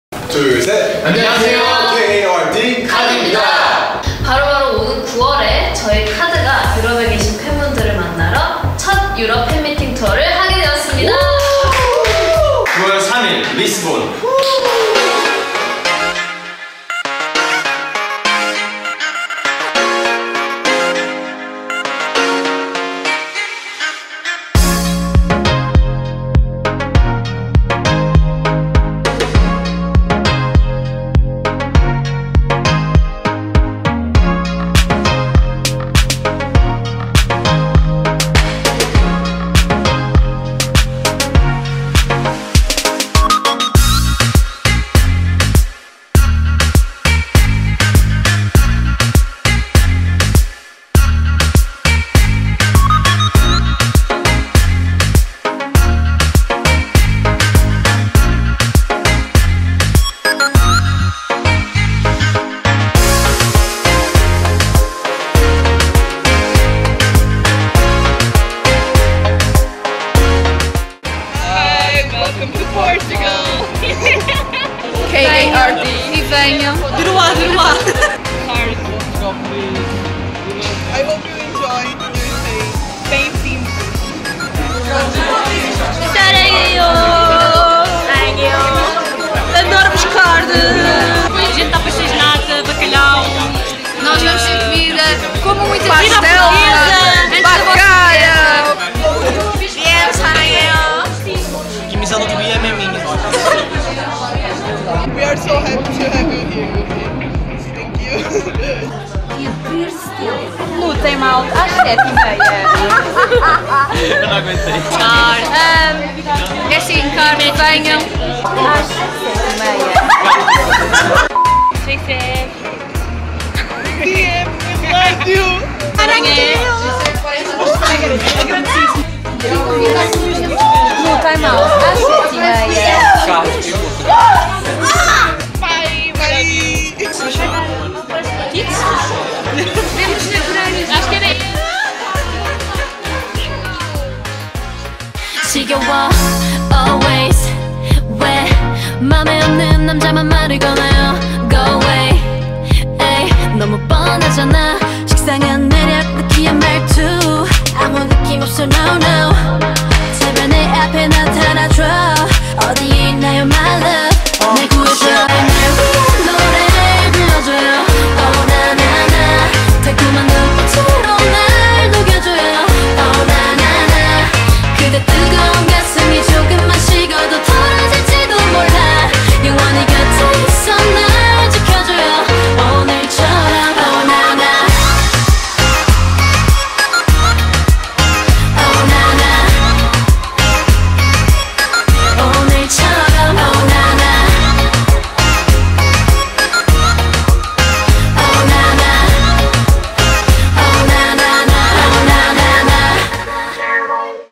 Countdown to K.A.R.D in Portugal!
K.A.R.D will be the 5th K-POP group doing a concert in Portugal!!! ;)
To show that the portuguese hidden kards are excited, we gathered some messages and put them in this video :)
Thank you all for participating! :D
And thank you to MyMusicTaste, K.A.R.D, DSP Media Entertainment and Time Out! :)

K.A.R.D we are waiting for you! See you in 5 days! :D

The concert will be on september 3rd at Time Out, Lisbon.
If you didn't buy a ticket, you still have time to do it! Don't miss the chance to see this special group and "live" a K-POP fanmeeting in PORTUGAL!